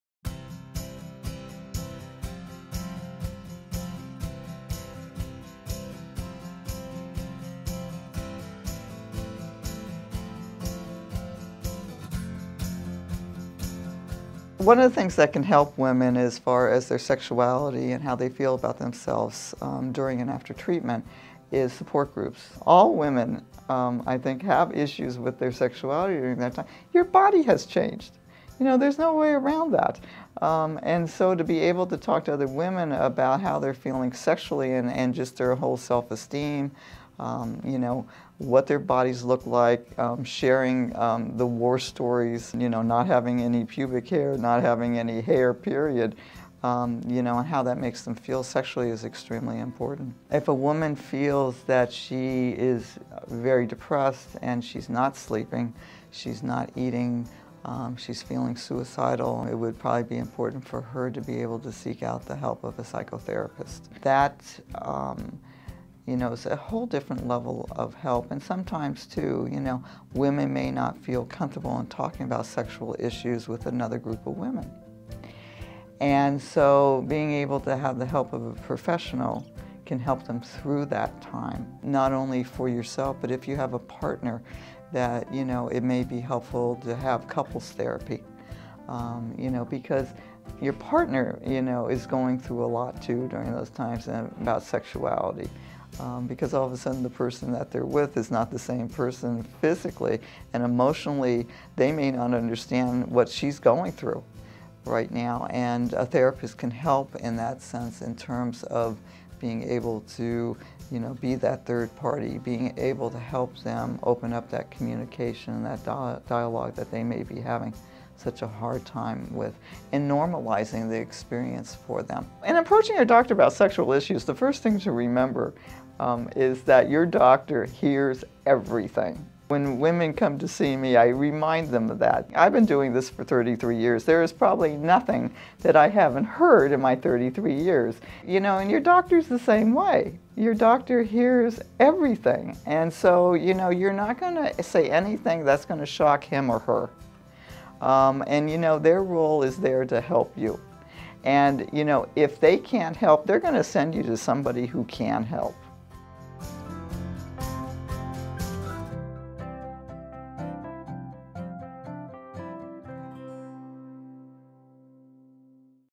Breast Cancer and Sexuality: Finding Support
Breast cancer impacts many facets of your life, including your sexuality. Psychotherapist and breast cancer survivor Elaine Franzetti, LCSW, discusses some options to help women find support.